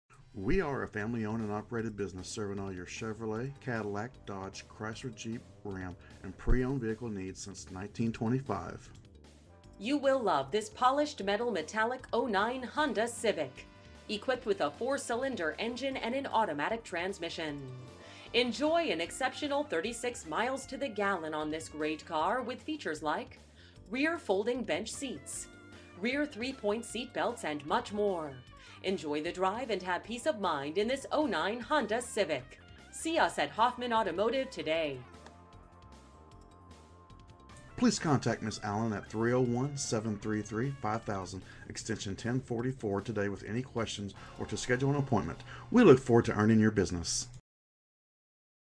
2009 Honda Civic Hoffman Automotive Hagerstown, MD 21740
Honda Civic Check out this Polished Metal Metallic Honda Civic Sedan, equipped with a 4 Cylinder engine and an automatic transmission.
MILES: 58,926
VIN:2HGFA16599H368672

Hoffman Automotive
101 South Edgewood Drive
Hagerstown, Maryland 21740